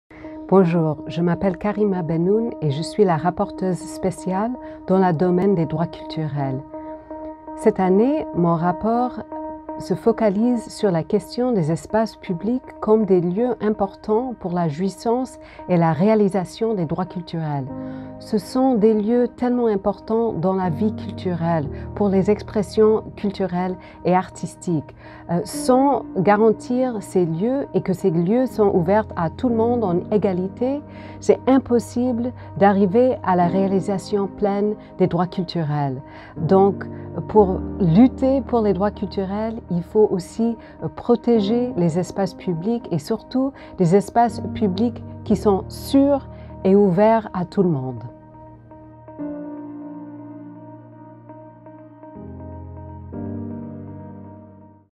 « C’est dans les espaces publics que la vie culturelle prend forme »
C’est dans les espaces publics que la vie culturelle se forme, se développe et s’exprime. Ils sont nécessaires pour l’exercice de nombreux droits humains.

C’est ce qu’affirme la Rapporteuse spéciale des Nations Unies dans le domaine des droits culturels, Karima Bennoune.